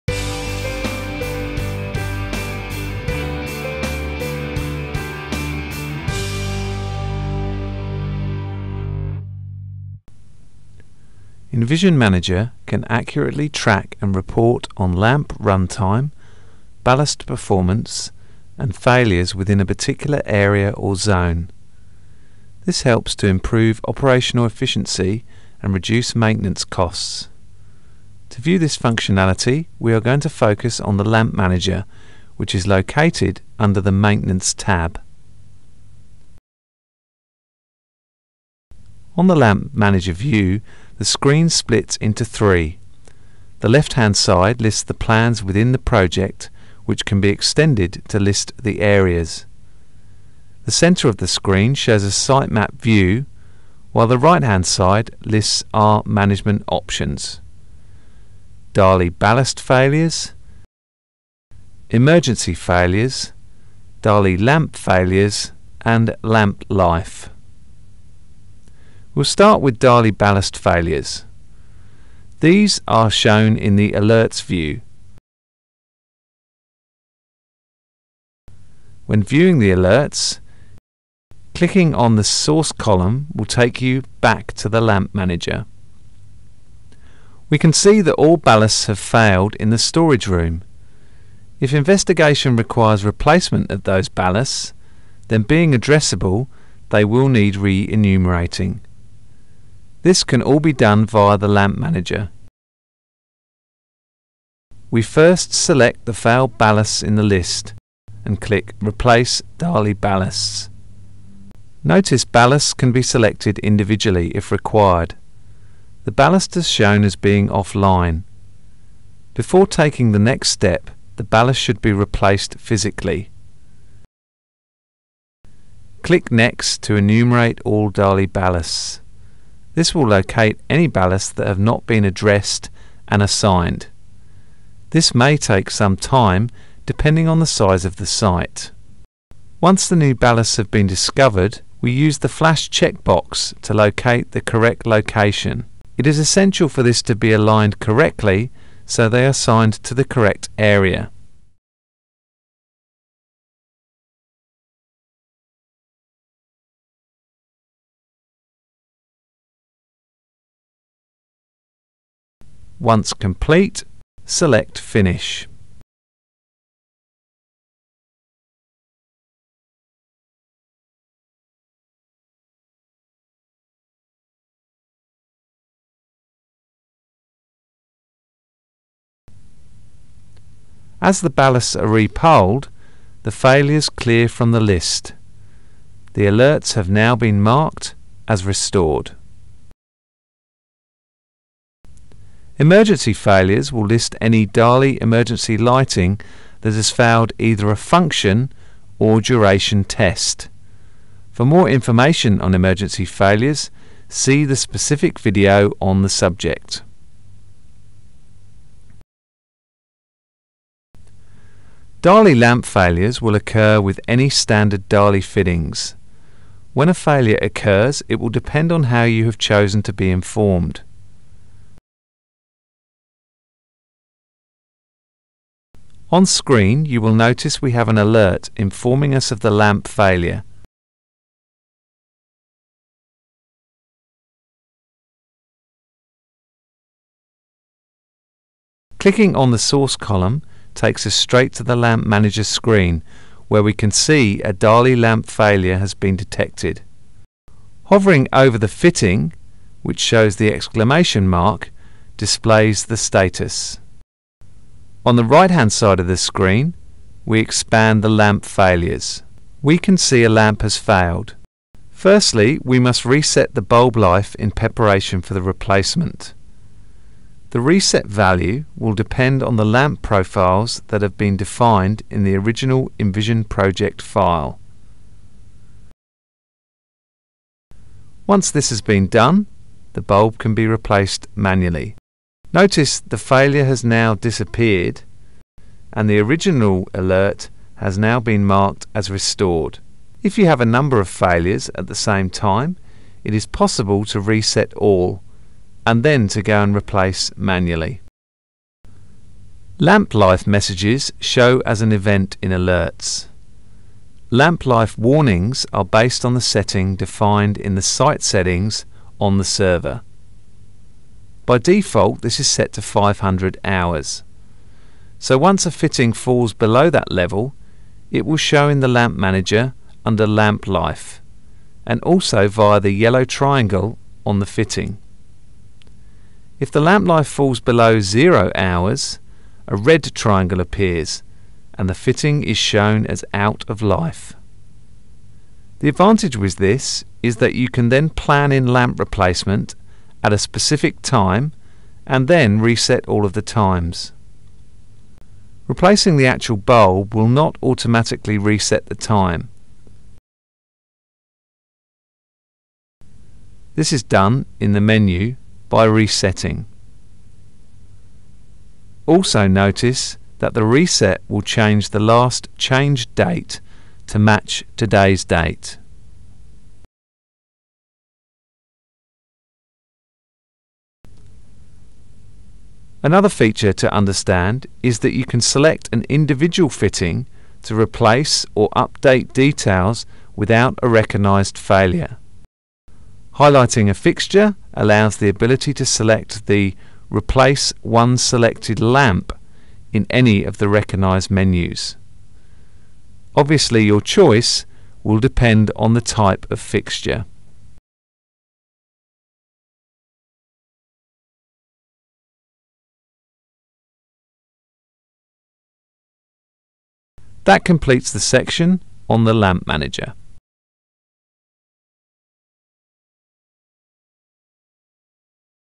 EnvisionManager Client - Maintenance: Lamp Manager
Learn how to use the lamp manager feature on the EnvisionManager client when dealing with situations like DALI ballast and lamp failures.